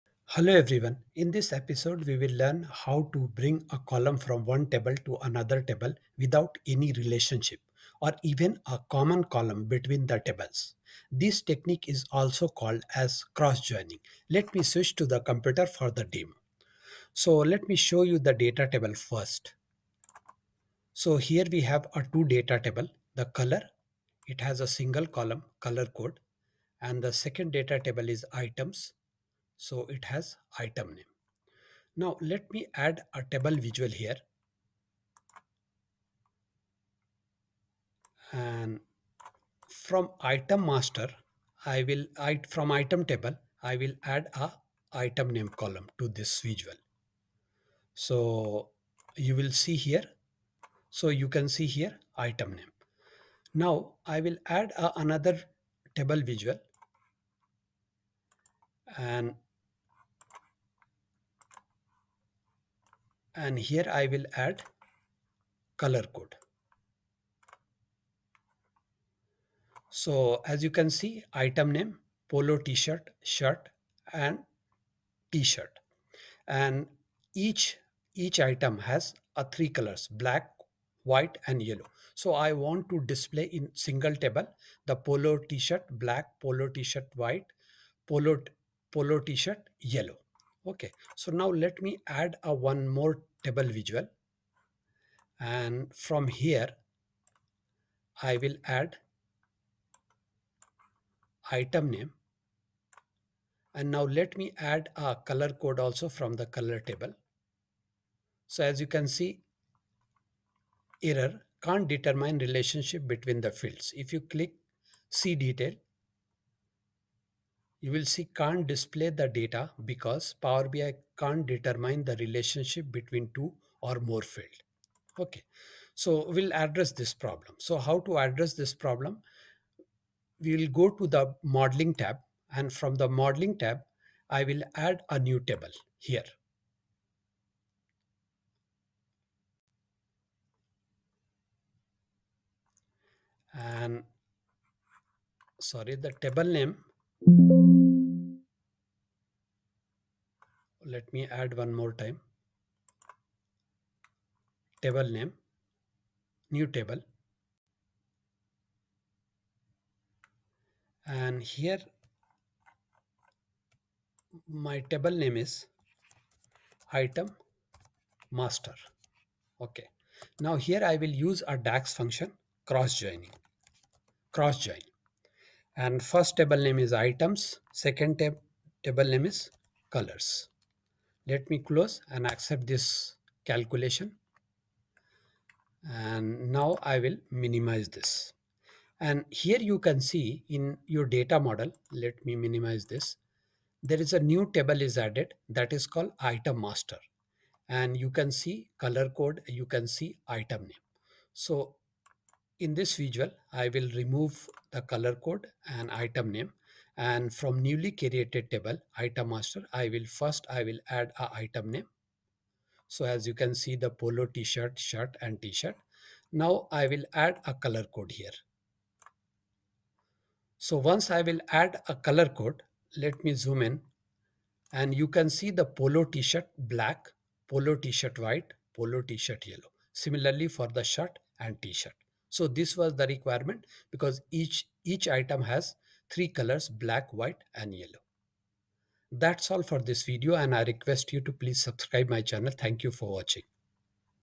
Cross Join in Power BI
How to bring a column from one table to another table without a relationship or common column between the tables. Step by step guide.